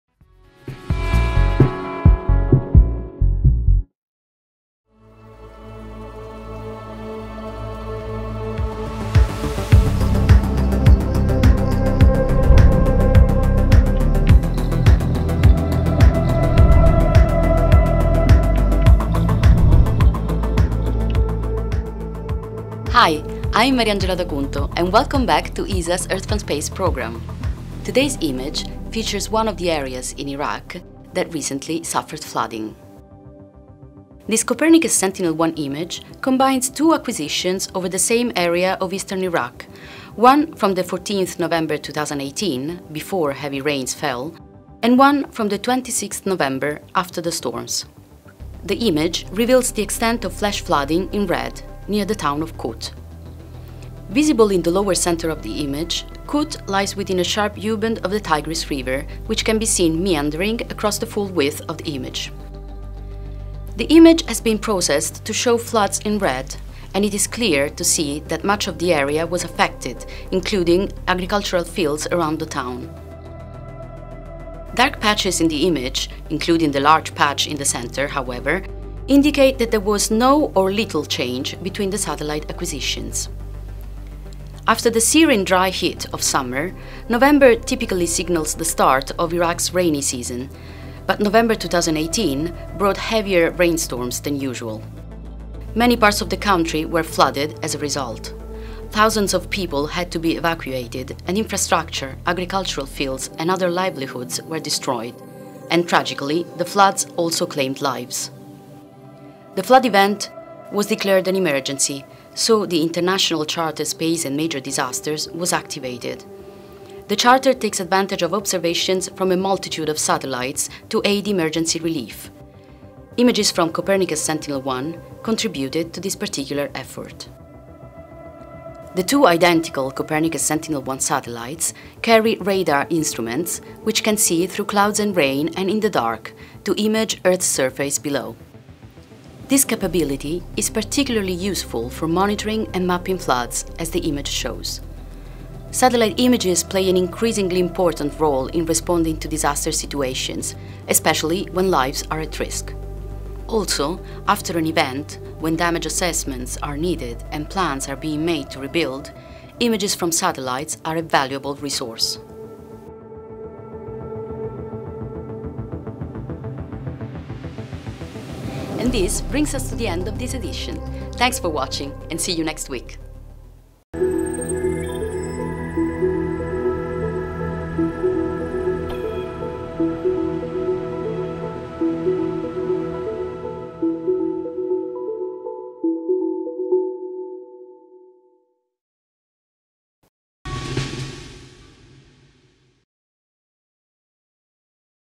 Earth from space: Iraq flood
The Copernicus Sentinel-1 mission takes us over one of the areas in Iraq that suffered flooding recently in this week's edition of the EarthfromSpace programme.